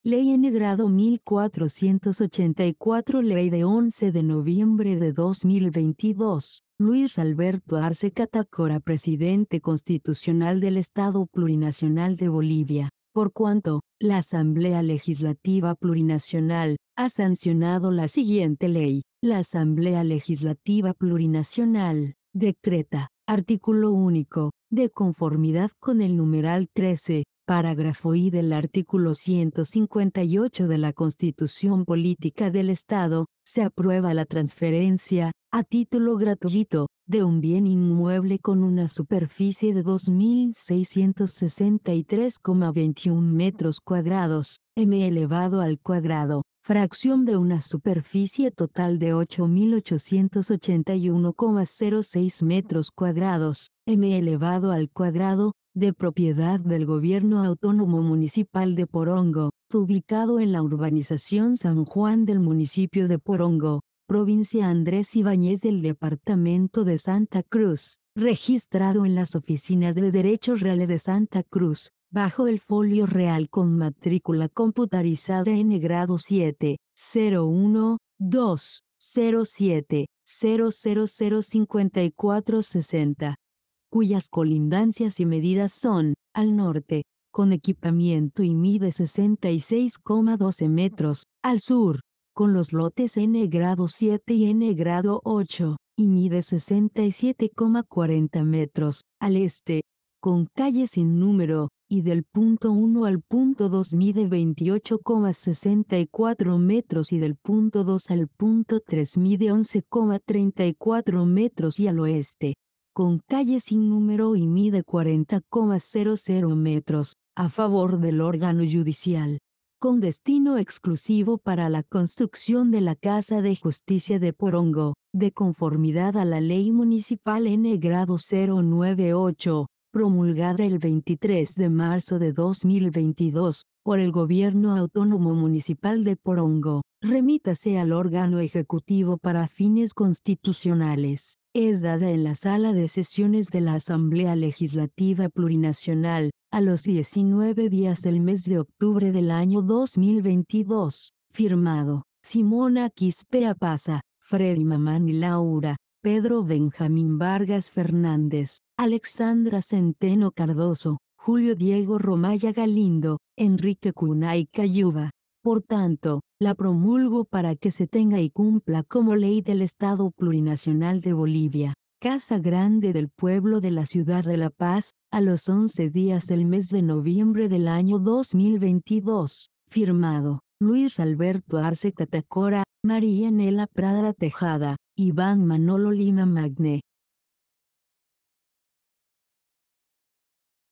LEY N° 1484 - Transferencia, de inmueble propiedad del GAM Porongo a favor del Órgano Judicial
DECRETA :

ARTÍCULO ÚNICO.

De conformidad con el numeral 13, Parágrafo I del Artículo 158 de la Constitución Política del Estado, se aprueba la transferencia, a título gratuito, de un bien inmueble con una superficie de 2.663,21 metros cuadrados (m²), fracción de una superficie total de 8.881,06 metros cuadrados (m²), de propiedad del Gobierno Autónomo Municipal de Porongo, ubicado en la Urbanización San Juan del Municipio de Porongo, Provincia Andrés Ibañez del Departamento de Santa Cruz, registrado en las oficinas de Derechos Reales de Santa Cruz, bajo el Folio Real con Matrícula Computarizada N° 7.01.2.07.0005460, cuyas colindancias y medidas son: al Norte, con equipamiento y mide 66,12 metros; al Sur, con los lotes N° 7 y N° 8, y mide 67,40 metros; al Este, con calle S/N, y del punto 1 al punto 2 mide 28,64 metros y del punto 2 al punto 3 mide 11,34 metros y al Oeste, con calle S/N y mide 40,00 metros; a favor del Órgano Judicial, con destino exclusivo para la Construcción de la Casa de Justicia de Porongo, de conformidad a la Ley Municipal N° 098, promulgada el 23 de marzo de 2022, por el Gobierno Autónomo Municipal de Porongo.